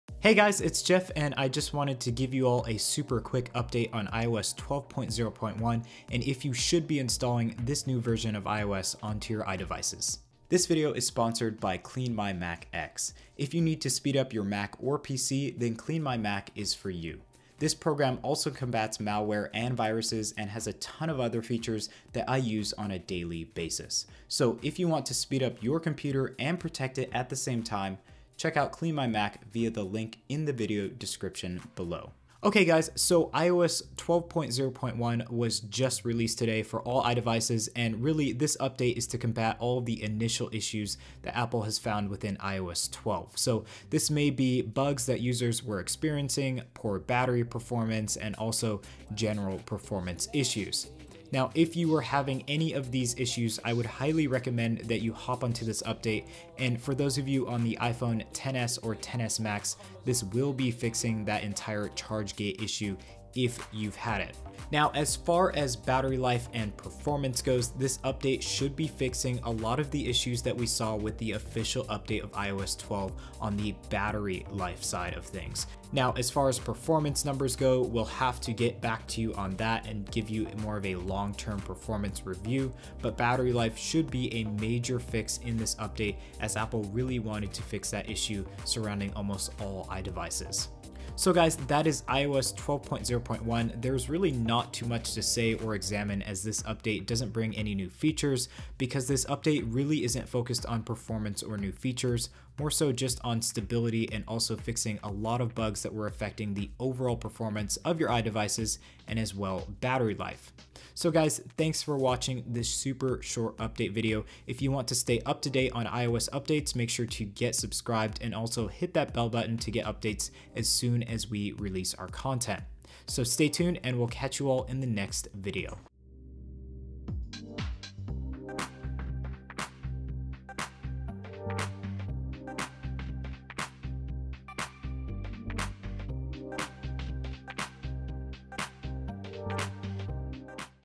iOS 12.0.1 Released! Major Bug Fixes!
Apple released iOS 12.0.1 this morning to the public which is an update to combat all of the issues or bugs found in iOS 12. This is a MUST INSTALL update for all of you and in this video we go over some reasons why.

Check out all of the gear that we use!